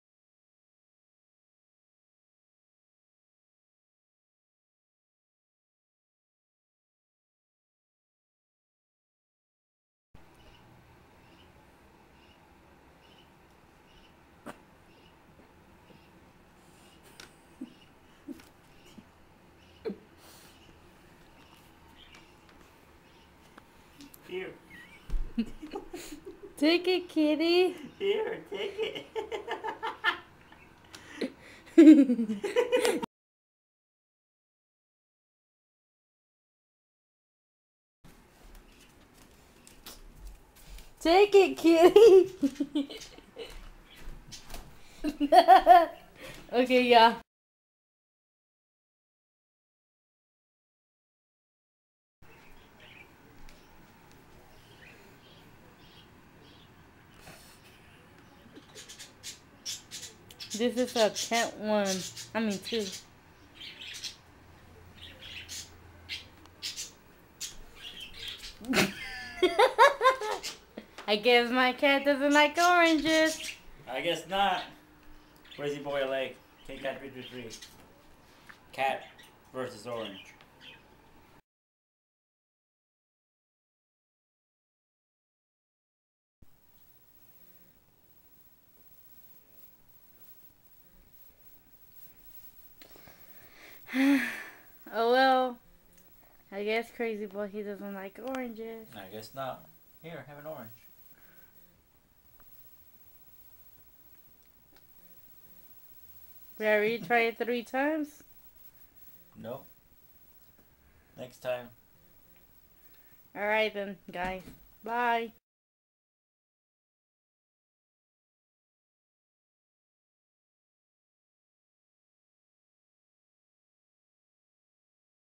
Our Cat Prince Hates Oranges :(
We Tried 4 times to see if our cat Loves Oranges. But I guess not He was running away and going under the bed When we were trying to give him oranges.